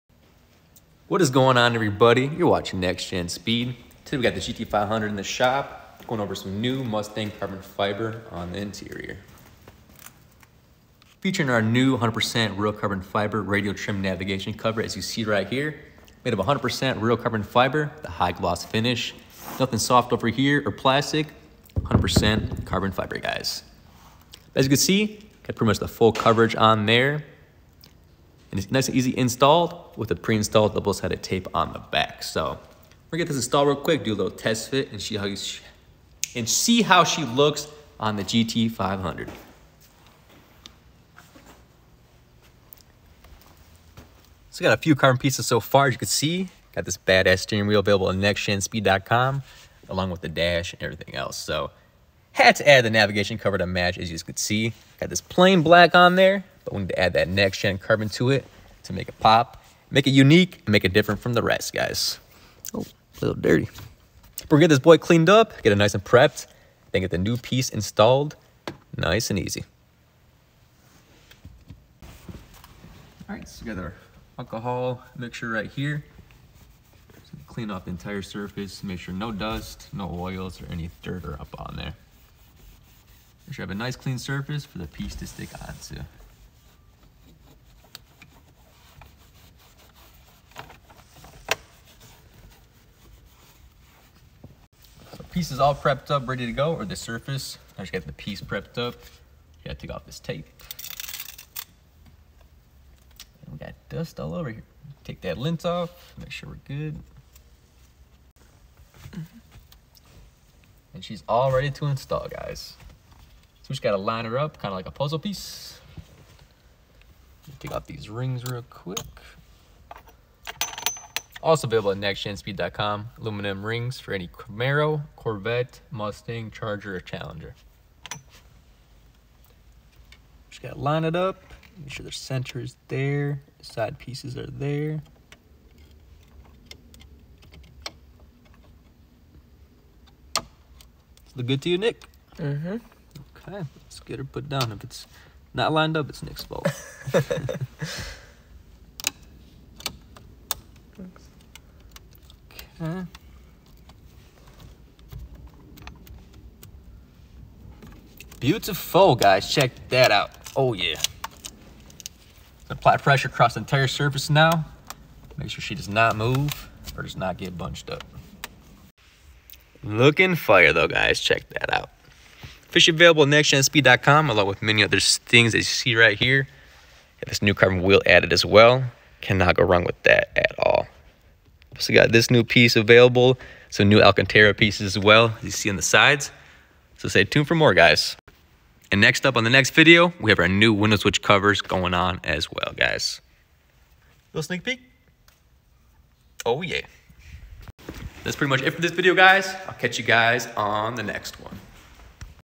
2015-23 Mustang Interior Mods: Radio/Nav Carbon Fiber Trim Cover by Next-Gen Speed
Going over our newly released high quality Radio/Navigation trim cover, available

▶Automotive Styling, Lighting, Performance, & More!